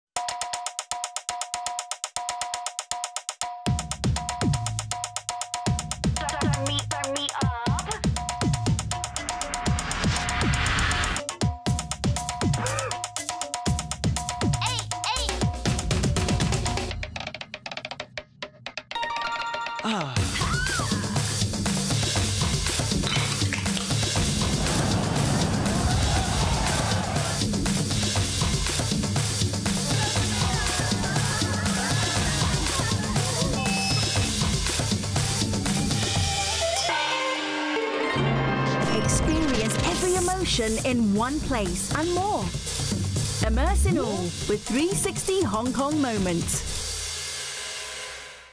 360 Hong Kong Moments – Experience every emotion in Hong Kong
[360 Hong Kong Moments] From harbour cruises and wellness activities, to karaoke and traditional Chinese operas, Hong Kong has something for everyone. Immerse yourself in all sorts of entertainment Hong Kong has to offer and be ready to experience every emotion like never before! Follow us for more upcoming episodes.